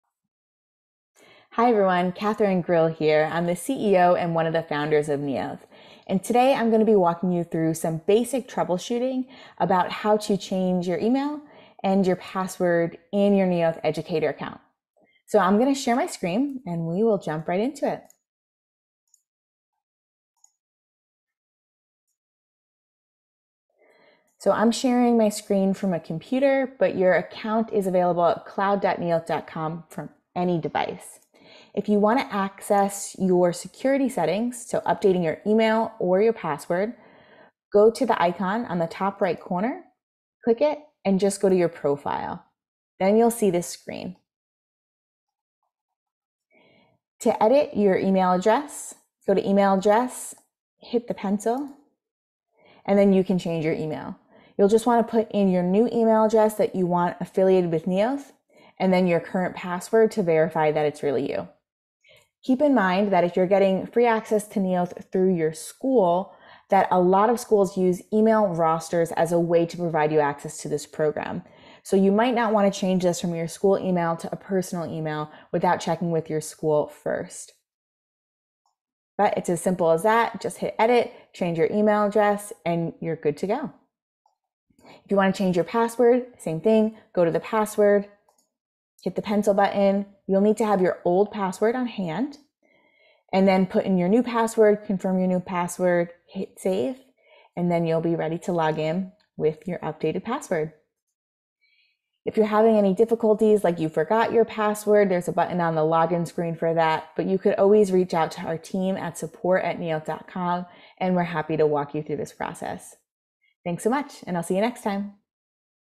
Teachers: How to change your email and password on Neolth
This quick video shows you how to change your email and/or password on Neolth. This video features Neolth's educator platform for teachers.

0:00 Intro
0:23 Access security settings
0:46 Change email
1:24 Change password
1:42 Contact tech support